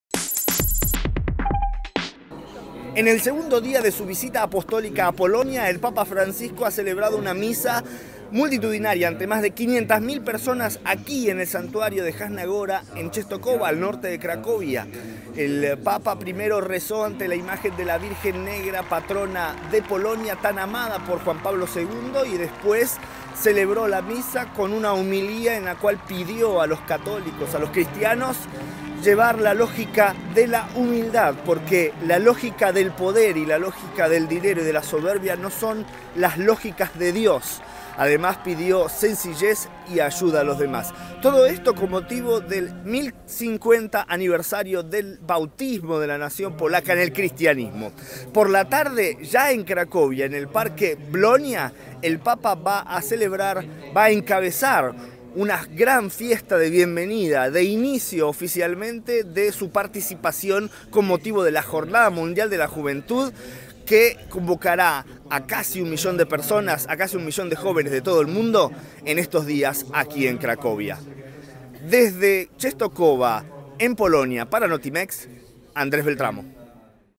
Desde Polonia el Papa advierte contra deseo de poder y grandeza
En su visita al santuario de la virgen patrona de Polonia, el Papa advirtió contra deseos "trágicamente humanos"